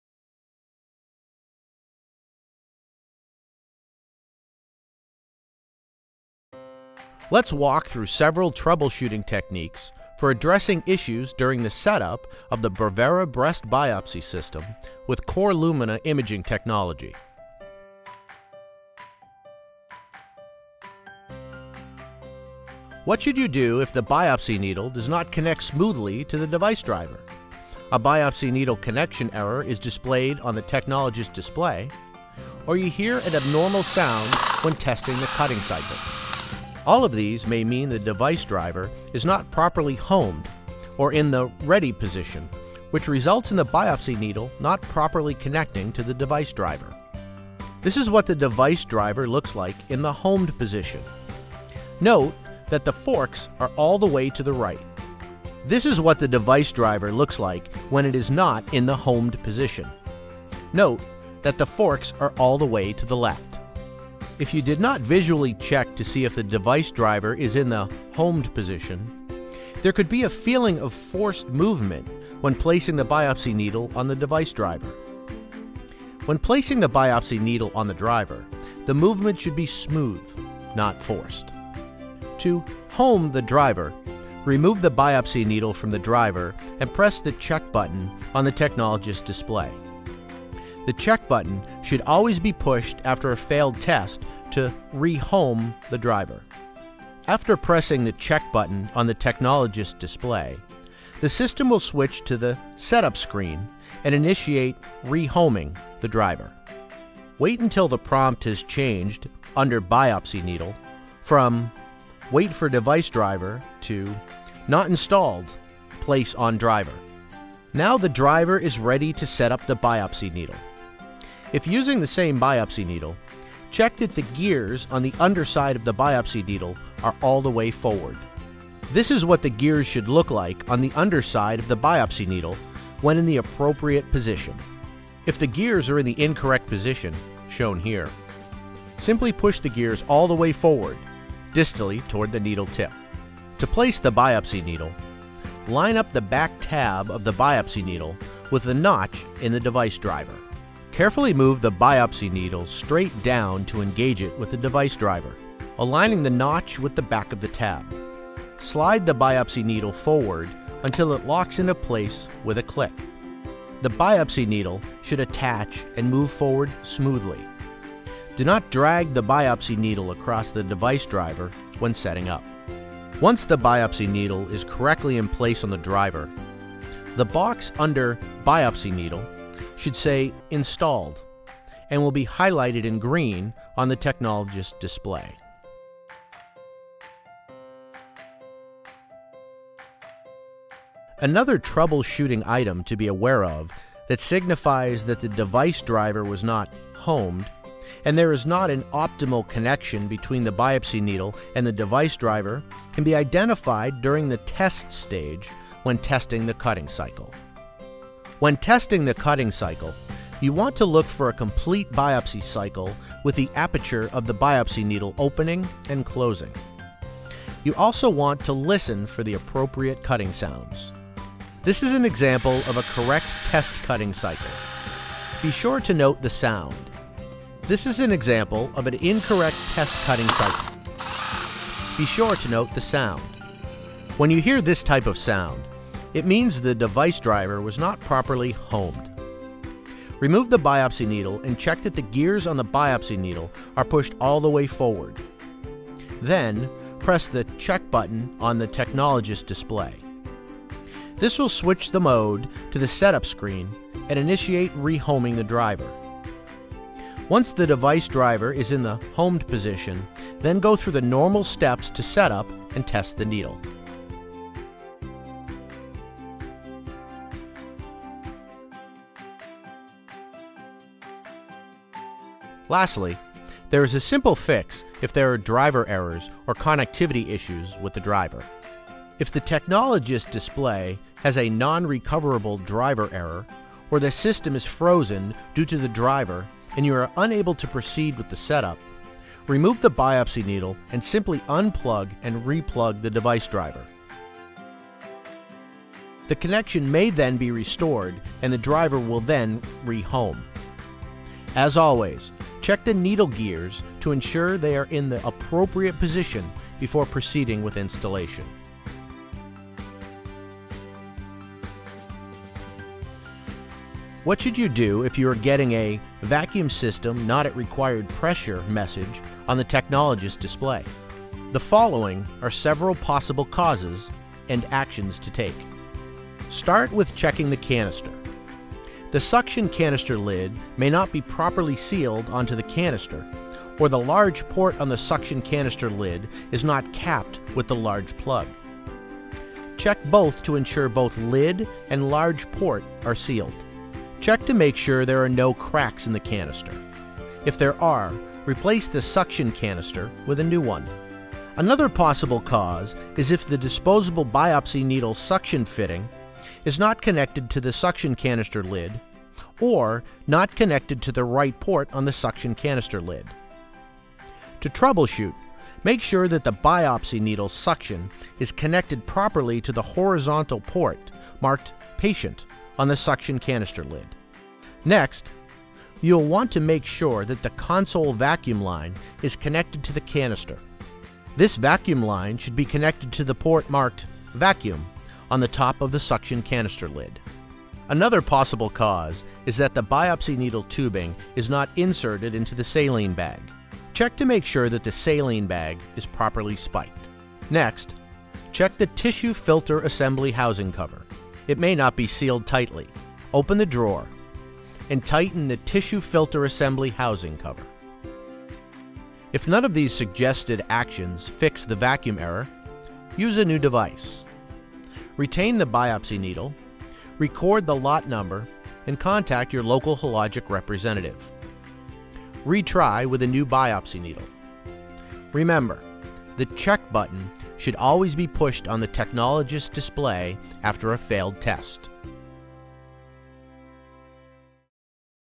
Brevera® Breast Biopsy System Training: Troubleshooting
Troubleshooting instructions for the Brevera Breast Biopsy System.